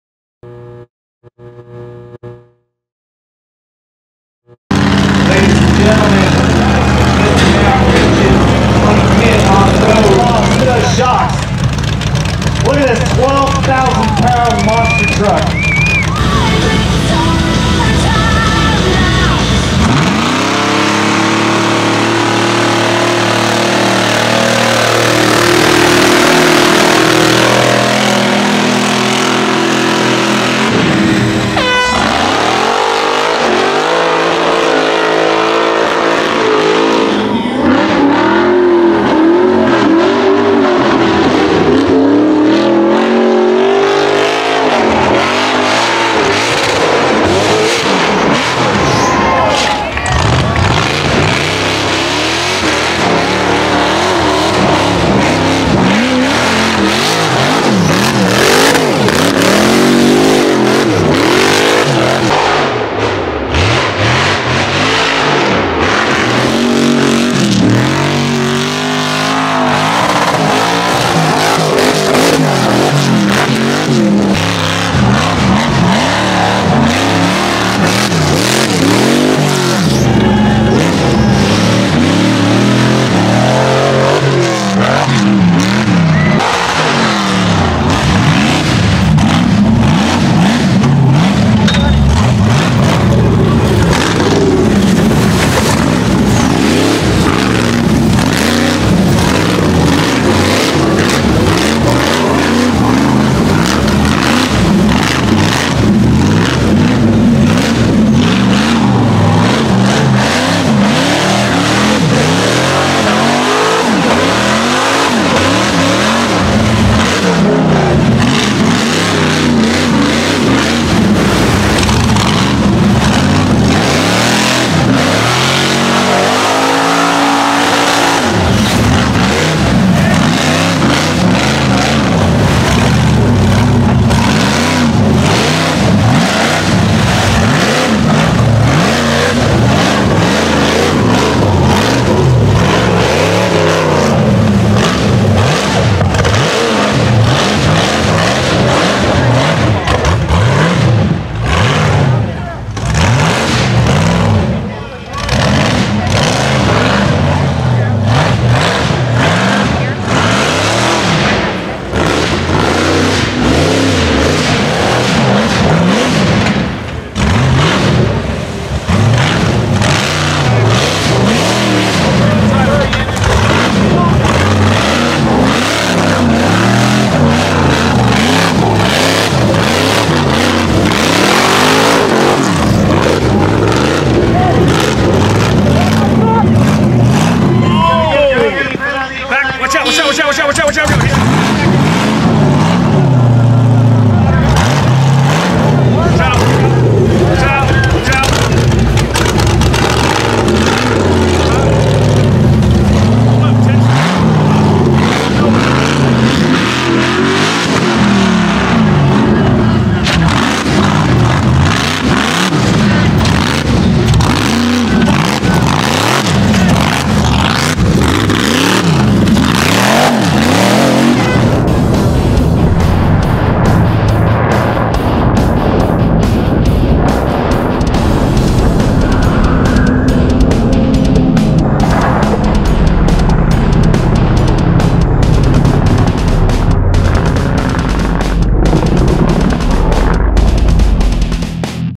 1000hp WARTHOG BUGGY aka FOSS HOGG MOUNTAIN HAVOC 2017
Bryan Foss hammers on the Warthog Buggy that he has rebuilt and renamed Foss Hogg at the 2017 Mountain Havoc 4x4 Madness event. Foss Hogg features a LS stroked 427 with a Whipple making 14 pounds Of boost. This helps it put down 1003 hp and 900ft lbs of torque at just 3000 rpm. The event takes place at Purcell Trench Ranch in Bonner's Ferry, Idaho and draws a ton of Top Truck Challenge style builds from all over the US and Canada. Mountain Mafia Entertainment puts on the event and the TTC drivers put on a great show on a multitude of off road courses.